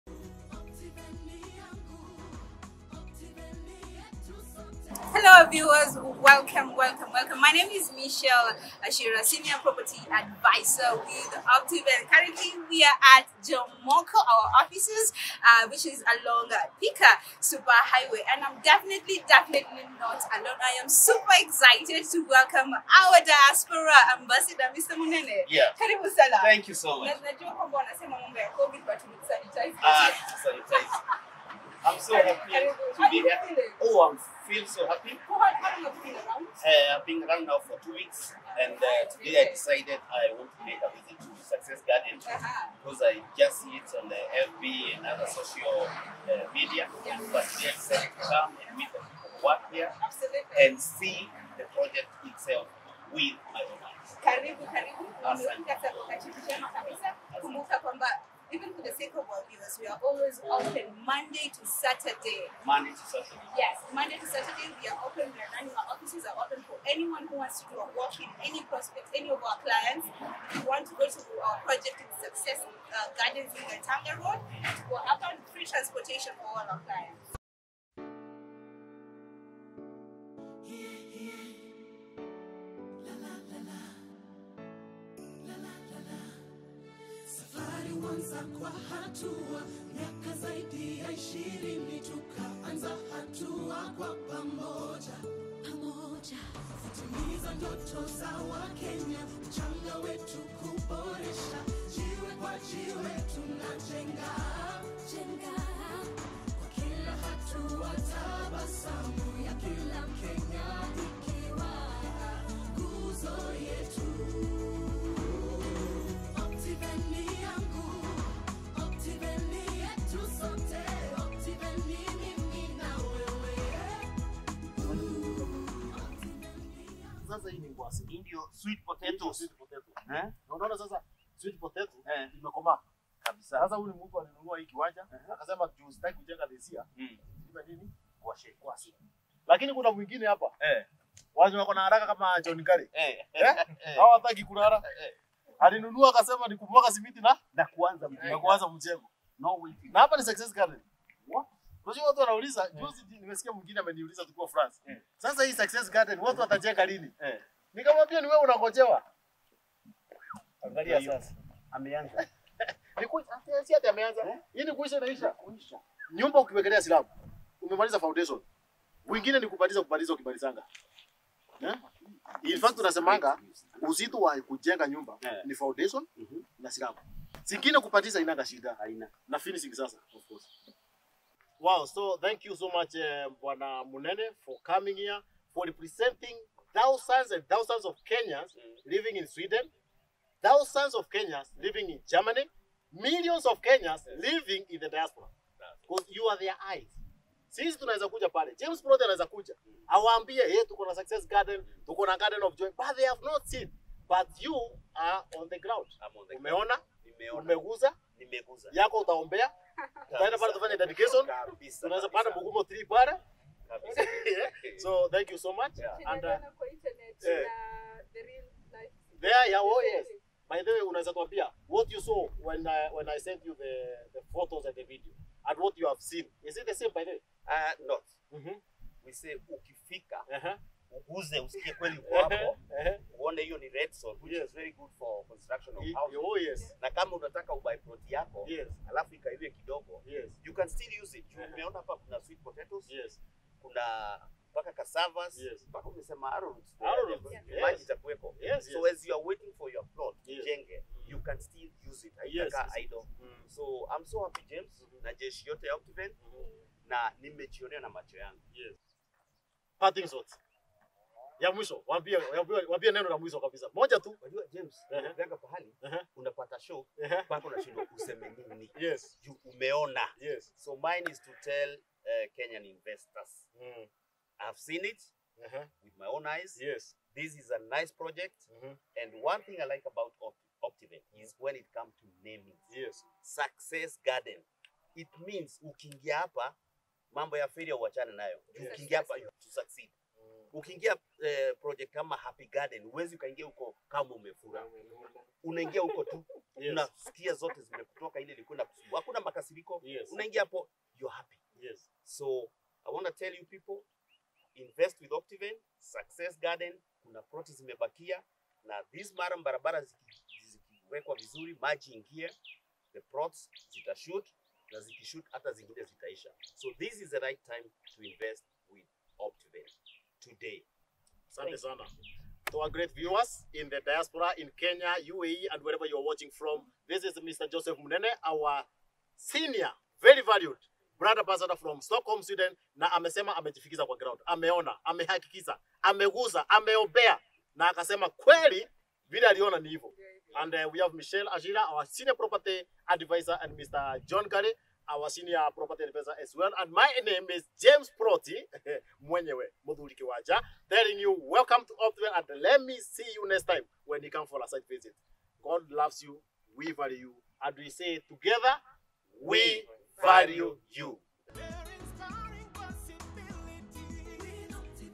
Ambassador Munene Visits Success Gardens
Joseph Munene, the Optiven Ambassador in Sweden, visited Success Gardens along Gatanga Road to check out its progress and he appreciated the amazing topography with the magnificent gate coming up very well.

Invest today by calling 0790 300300